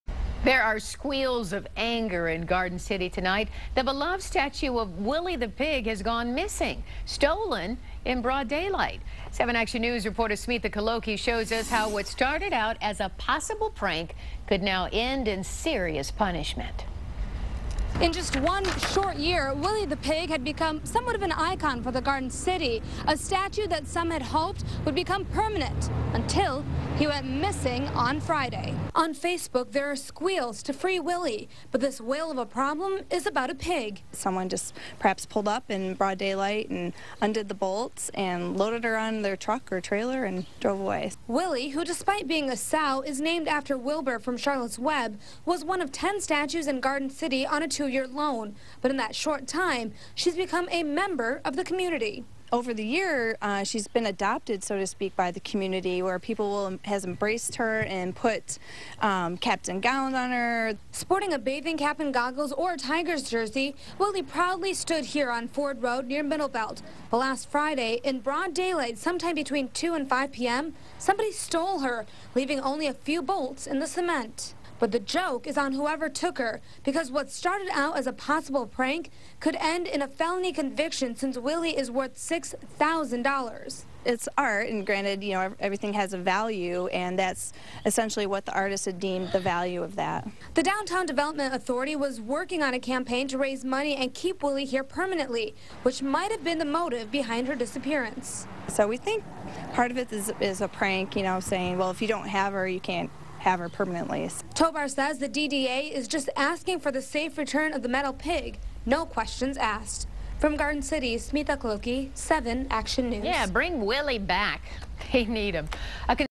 Pig stolen in Garden City
Police are searching for a pig that was stole in Garden City.